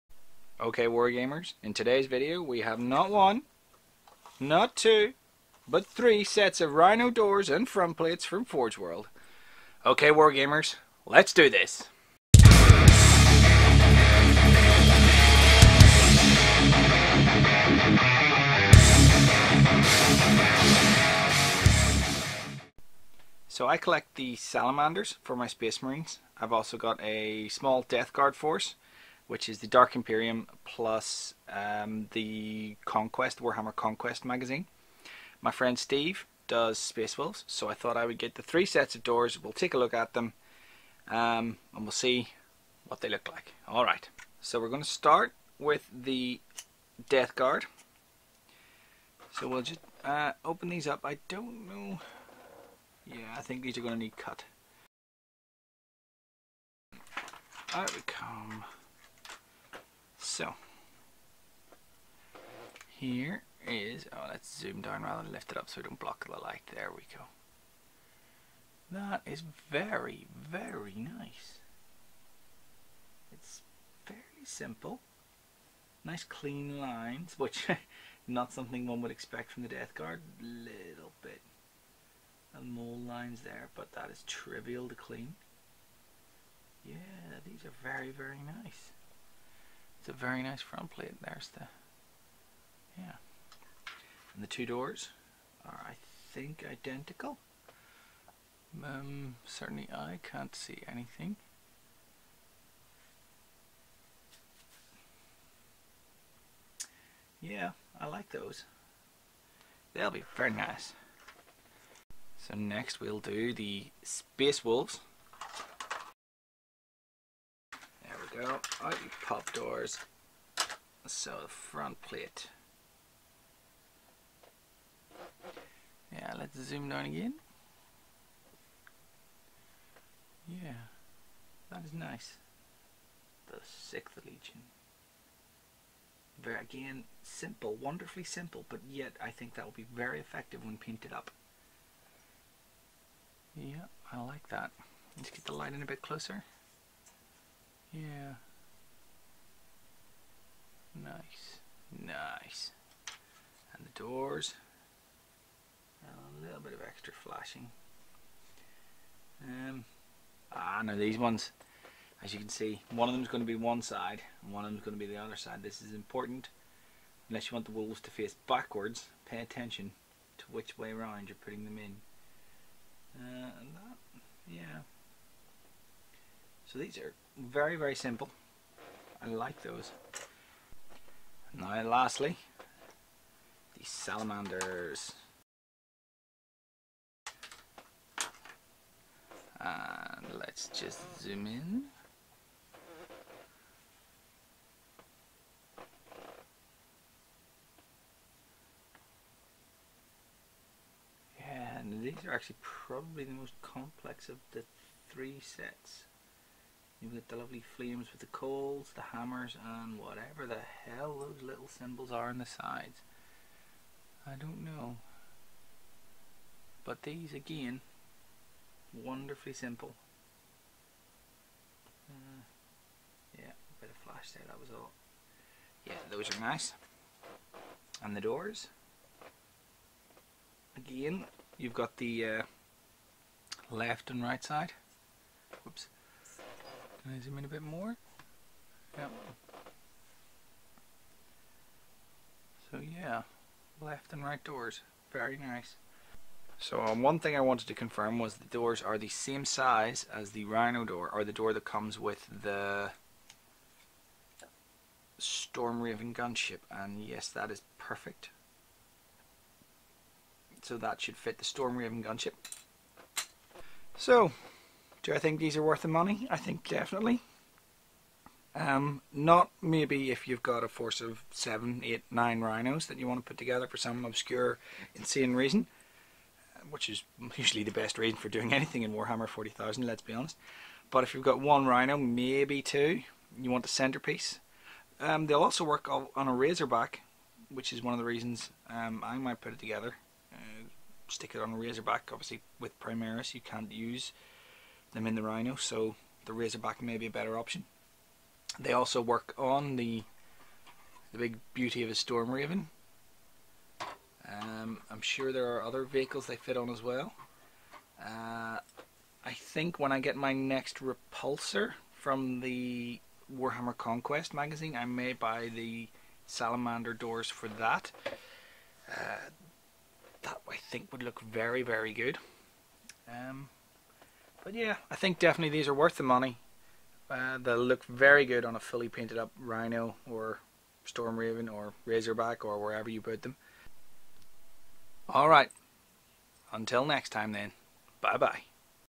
Forgeworld Rhino Doors - Death Gurad. Space Wolves and Salamanders.
Lets take a look at the Rhino doors for the Death Guard, the Space Wolves and the Salamanders. These sets are £14 each from Forgeworld.
Are the worth the money? In short; yes.

They're not just good, they're good enough...